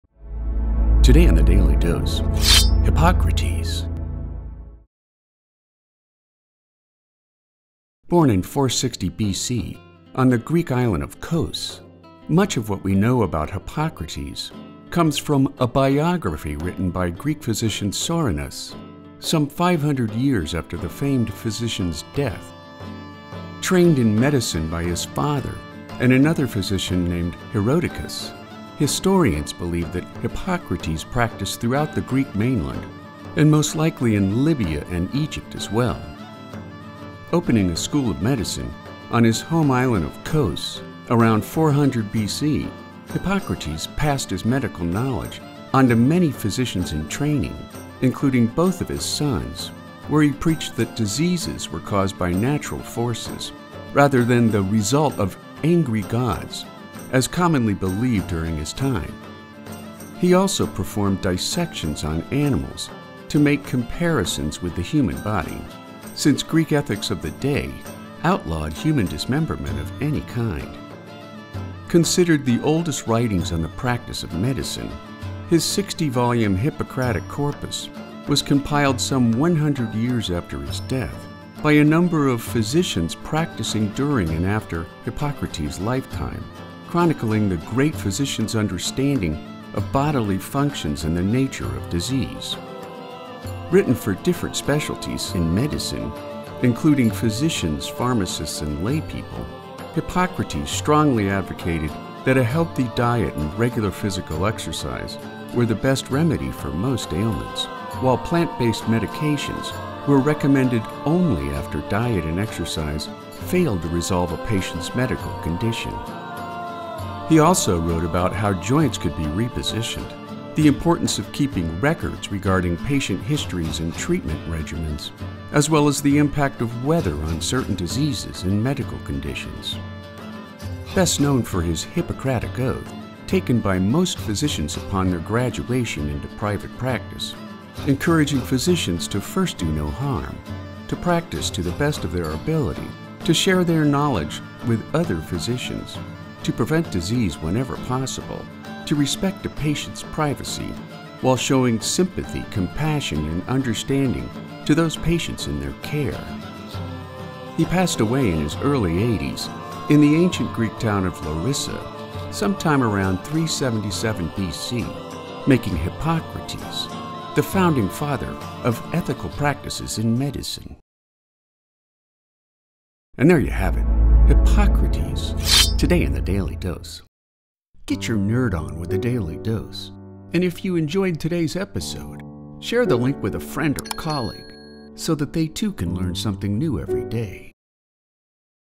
Hippocrates: Father of Medical Ethics and Practice
Today's Daily Dose short biography film covers Hippocrates, who is known as the founding father of modern medical ethics and practice. The filmmaker has included the original voice over script to further assist your understanding:

Today on The Daily Dose, Hippocrates.

Born in 460 BC on the Greek island of Kos, much of what we know about Hippocrates comes from a biography written by Greek physician Soranus, some 500 years after the famed physician’s death. Trained in medicine by his father and another physician named Herodicus, historians believe that Hippocrates practiced throughout the Greek mainland, and most likely in Libya and Egypt as well. Opening a school of medicine on his home island of Kos around 400 BC, Hippocrates passed his medical knowledge onto many physicians-in-training, including both of his sons, where he preached that diseases were caused by natural forces, rather than the result of angry gods as commonly believed during his time. He also performed dissections on animals to make comparisons with the human body, since Greek ethics of the day outlawed human dismemberment of any kind.

Considered the oldest writings on the practice of medicine, his 60-volume Hippocratic Corpus was compiled some 100 years after his death, by a number of physicians practicing during and after Hippocrates’ lifetime, chronicling the great physician’s understanding of bodily functions and the nature of disease. Written for different specialties in medicine, including physicians, pharmacists and laypeople, Hippocrates strongly advocated that a healthy diet and regular physical exercise were the best remedy for most ailments, while plant-based medications were recommended only after diet and exercise failed to resolve a patient’s medical condition. He also wrote about how joints could be repositioned, the importance of keeping records regarding patient histories and treatment regimens, as well as the impact of weather on certain diseases and medical conditions.

Best known for his Hippocratic Oath—taken by most physicians upon their graduation into private practice— encouraging physicians to first do no harm, to practice to the best of their ability, to share their knowledge with other physicians, to prevent disease whenever possible, to respect a patient’s privacy, while showing sympathy, compassion and understanding to those patients in their care. He passed away in his early 80s in the ancient Greek town of Larissa, sometime around 377 BC, making Hippocrates, the founding father of ethical practices in medicine.

And there you have it, Hippocrates, today on The Daily Dose.